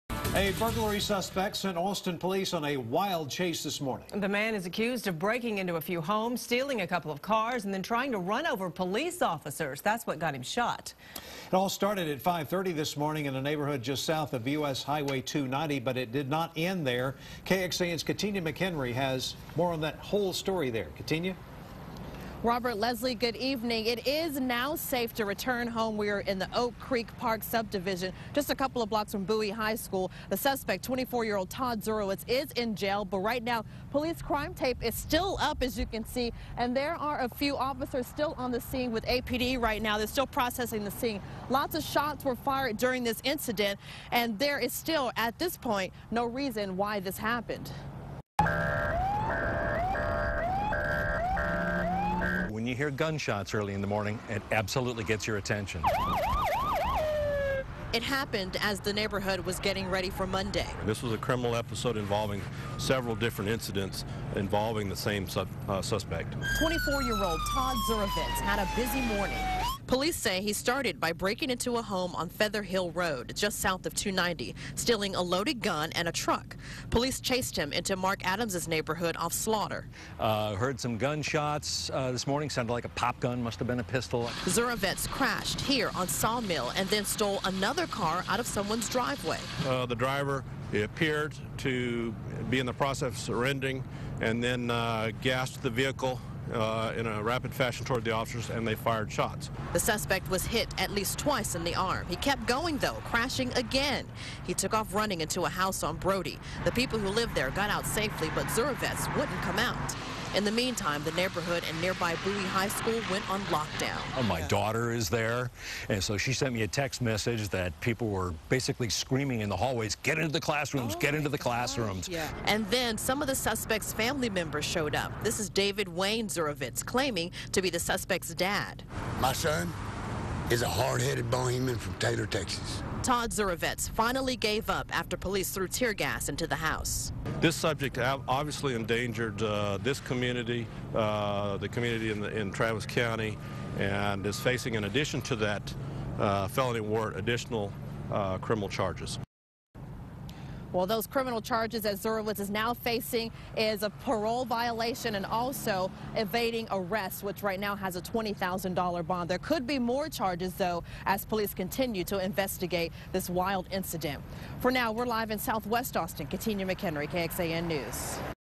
Multi-burglary suspect shot in the arm - 5 pm News
Following two stolen cars, three house burglaries and shots fired from police, authorities have arrested Todd Zurovitz after he barricaded himself inside a home on Brodie Lane.